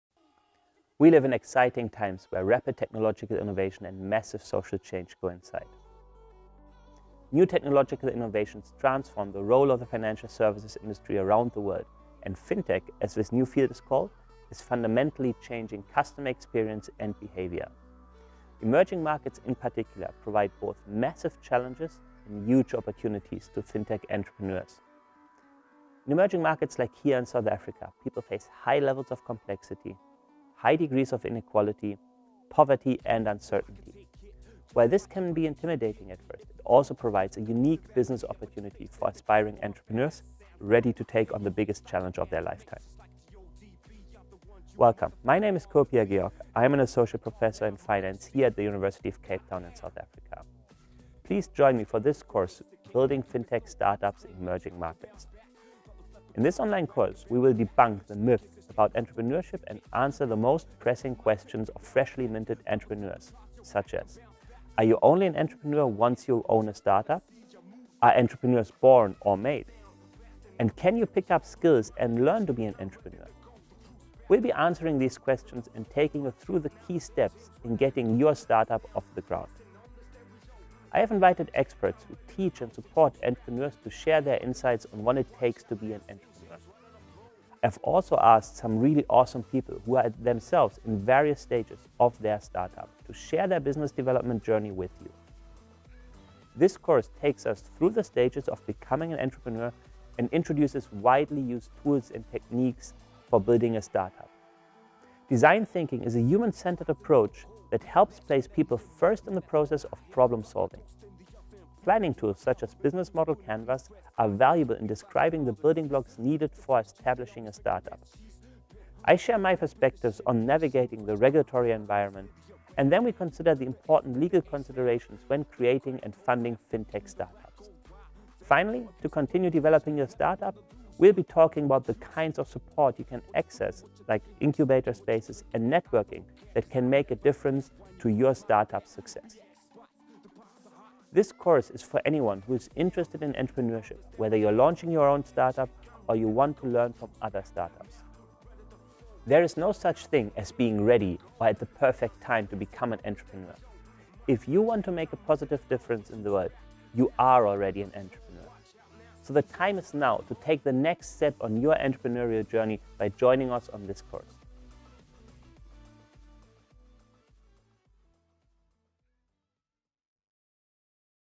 Building Fintech Startups in Emerging Markets Course Trailer
Join our online course Building Fintech Startups in Emerging Markets on Coursera. The course is open to all aspiring entrepreneurs.